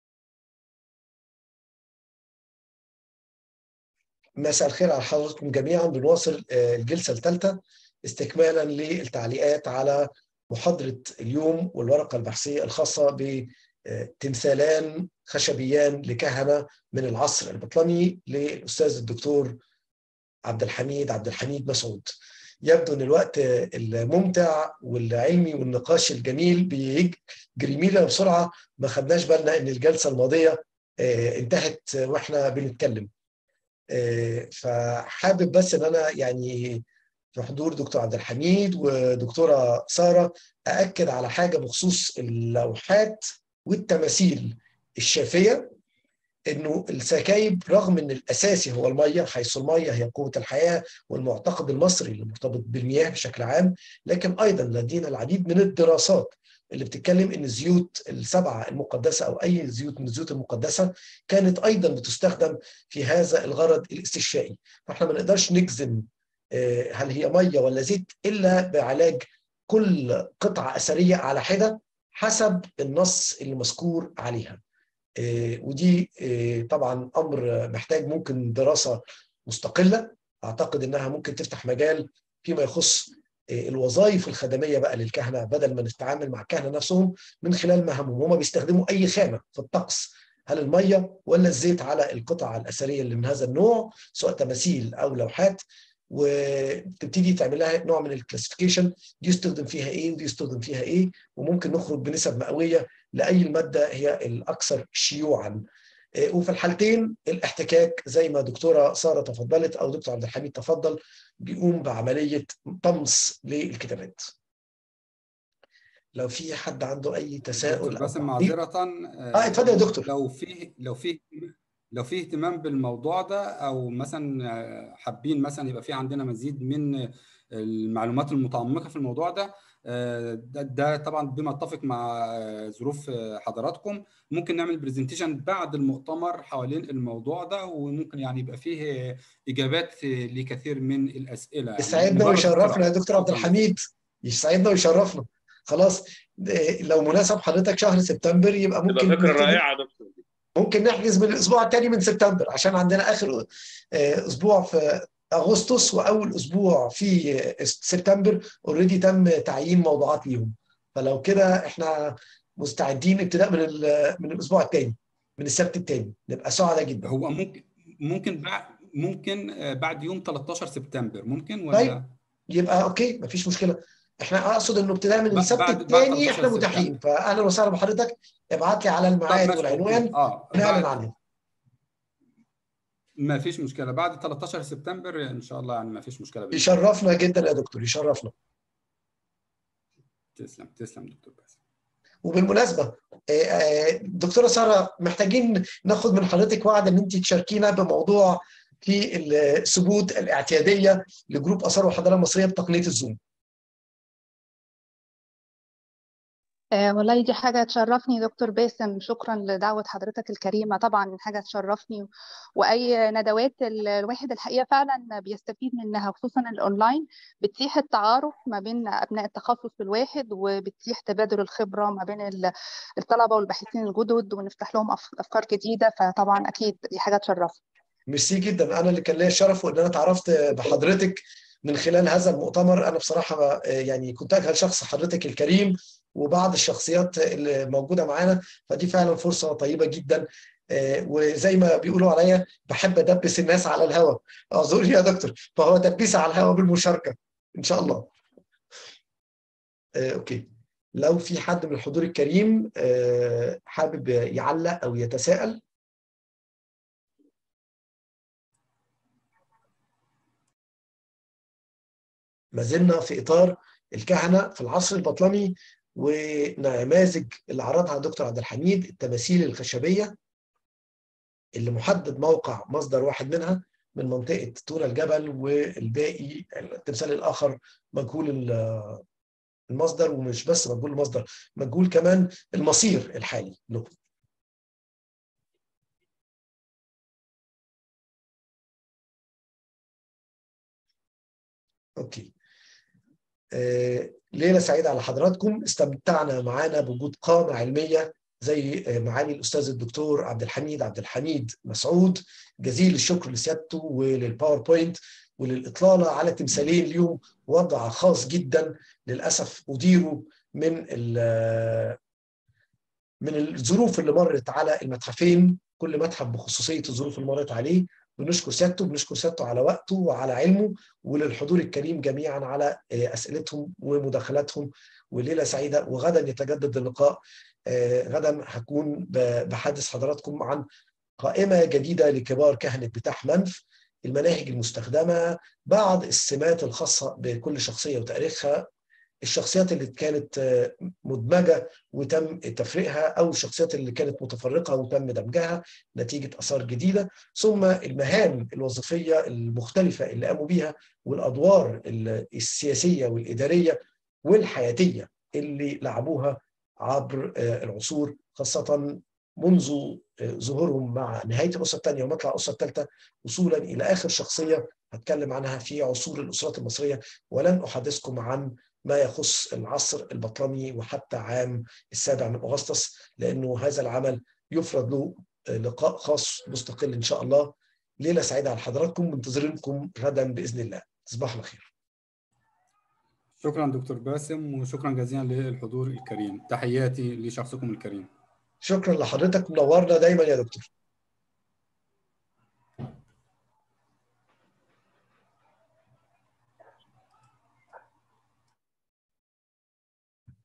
12. تمثالان لكهنة من العصر البطلمي (ج3/ 3) - أ.د. عبدالحميد عبدالحميد مسعود
12. تمثالان لكهنة من العصر البطلمي (ج3/ 3) - أ.د. عبدالحميد عبدالحميد مسعود

المقطع الثالث من ثلاثة يتضمن الأول والثاني المحاضرة وبقية الثاني والثالث يتضمنان المداخلات
ضمن المؤتمر الثاني لجروب آثار وحضارة مصرية على الفيسبوك
الكهنة والكهنوت في مصر القديمة حتى نهاية العصر البيزنطي